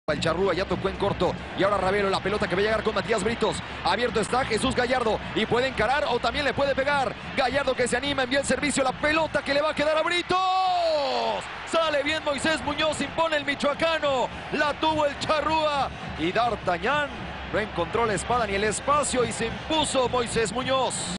Matías Britos desperdicia un mano a mano para el 2-0
Gran centro de Jesús Gallardo que el uruguayo desperdició ante la salida de Muñoz.